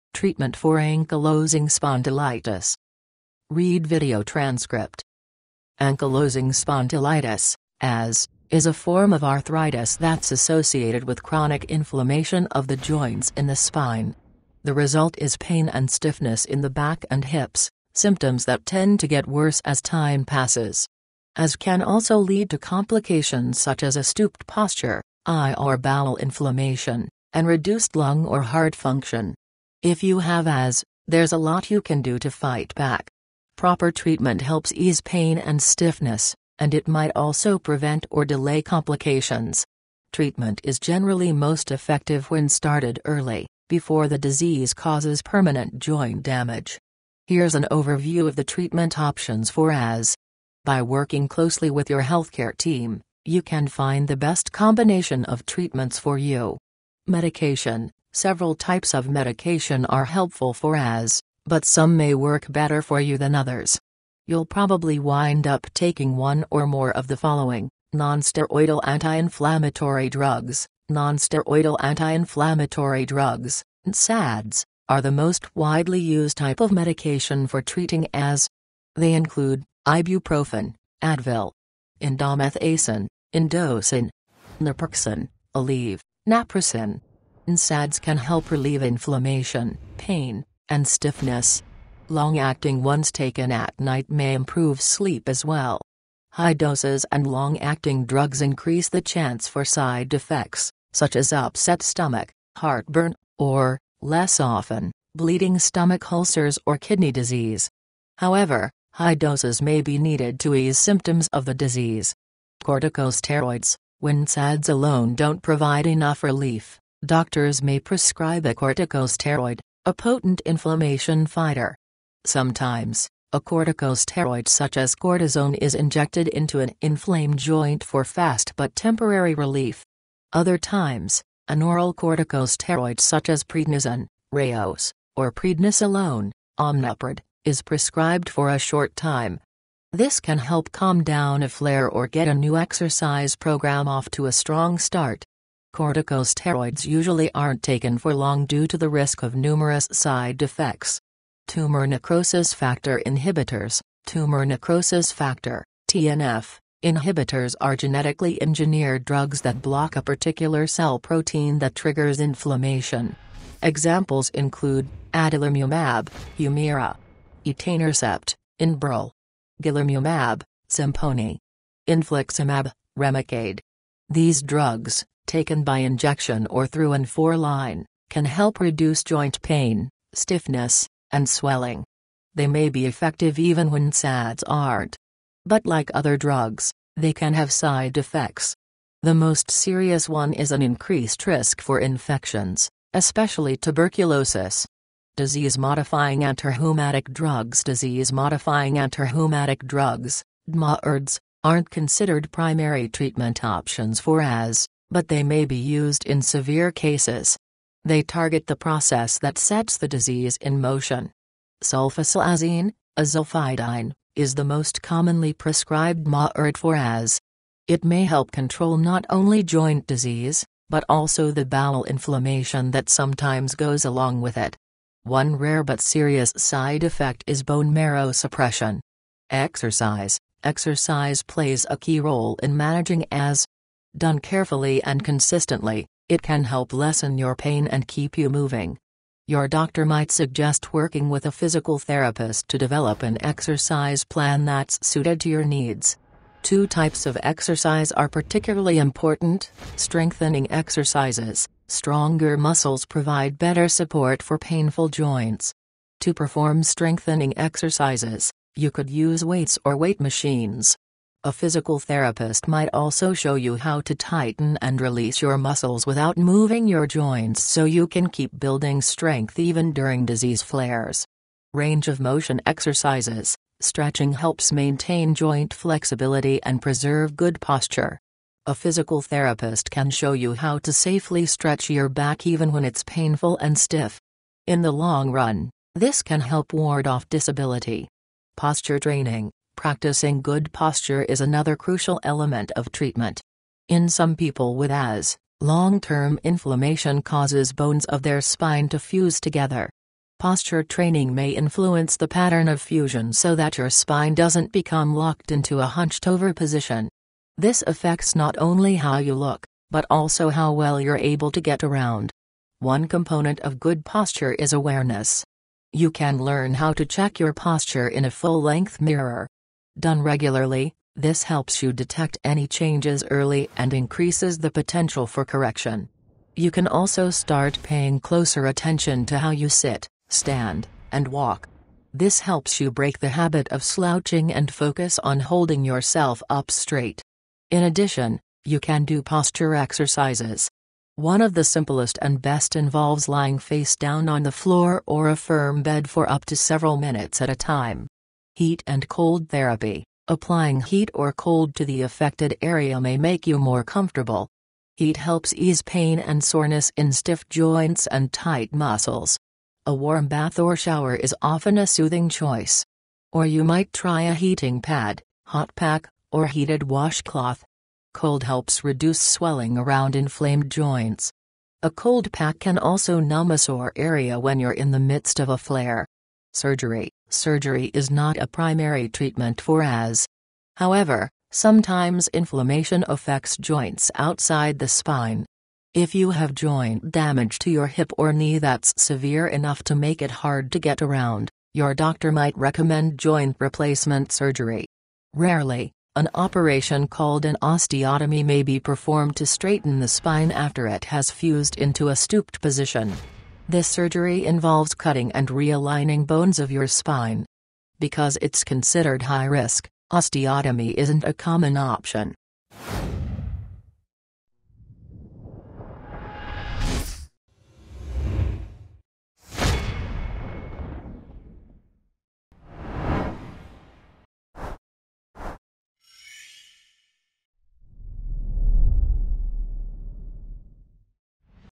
Causes Symptoms and Treatment of Ankylosing Spondylitis
Treatment for Ankylosing Spondylitis.

Read Video Transcript ».

Ankylosing spondylitis (AS) is a type of joint inflammation that is related with unending irritation of the joints in the spine. The outcome is torment and firmness in the back and hips — side effects that have a tendency to deteriorate over the long haul. AS can likewise prompt difficulties, for example, a stooped stance, eye or inside aggravation, and diminished lung or heart work.

In the event that you have AS, there's a considerable measure you can do to battle back. Legitimate treatment enables straightforwardness to torment and firmness, and it may likewise avert or defer confusions. Treatment is for the most part best when begun ahead of schedule, before the infection causes perpetual joint harm.

Here's an outline of the treatment alternatives for AS. By working intimately with your human services group, you can locate the best mix of medications for you.

Drug:

A few sorts of medicine are useful for AS, yet some may work preferable for you over others. You'll most likely breeze up taking at least one of the accompanying:

Nonsteroidal calming drugs:

Nonsteroidal calming drugs (NSAIDs) are the most generally utilized kind of medicine for regarding AS. They include:

*ibuprofen (Advil).

*indomethacin (Indocin).

*naproxen (Aleve, Naprosyn).

NSAIDs can help ease irritation, torment, and solidness. Long-acting ones taken during the evening may enhance rest also.

High dosages and long-acting medications increment the possibility for reactions, for example, furious stomach, acid reflux, or, less regularly, draining stomach ulcers or kidney sickness. Be that as it may, high measurements might be expected to ease indications of the malady.

Corticosteroids:

At the point when NSAIDs alone don't give enough help, specialists may endorse a corticosteroid, an intense aggravation contender.

Now and again, a corticosteroid, for example, cortisone is infused into a kindled joint for quick however brief help. Different circumstances, an oral corticosteroid, for example, prednisone (Rayos) or prednisolone (Omnipred) is recommended for a brief span. This can enable quiet to down a flare or get another activity program off to a solid begin.

Corticosteroids more often than not aren't taken for long because of the danger of various symptoms.

Tumor corruption factor inhibitors:

Tumor corruption factor (TNF) inhibitors are hereditarily designed medications that square a specific cell protein that triggers aggravation. Cases include:

*adalimumab (Humira).

*etanercept (Enbrel).

*golimumab (Simponi).

*infliximab (Remicade).

These medications — taken by infusion or through an IV line — can help lessen joint agony, firmness, and swelling. They might be viable notwithstanding when NSAIDs aren't. In any case, as different medications, they can have reactions. The most genuine one is an expanded hazard for contaminations, particularly tuberculosis.

Sickness adjusting antirheumatic drugs

Sickness altering antirheumatic drugs (DMARDs) aren't viewed as essential treatment alternatives for AS, however they might be utilized as a part of serious cases. They focus on the procedure that gets the ailment under way.

Sulfasalazine (Azulfidine) is the most regularly endorsed DMARD for AS. It might help control joint ailment, as well as the gut aggravation that occasionally obliges it. One uncommon yet genuine reaction is bone marrow concealment.

Exercise:

Exercise assumes a key part in overseeing AS. Done painstakingly and reliably, it can help diminish your agony and keep you moving. Your specialist may propose working with a physical advisor to build up an activity arrange for that is suited to your requirements. Two kinds of activity are especially essential:

Fortifying activities:

More grounded muscles give better help to difficult joints. To perform reinforcing works out, you could utilize weights or weight machines. A physical specialist may likewise demonstrate to you generally accepted methods to fix and discharge your muscles without moving your joints so you can continue building quality notwithstanding amid infection flares.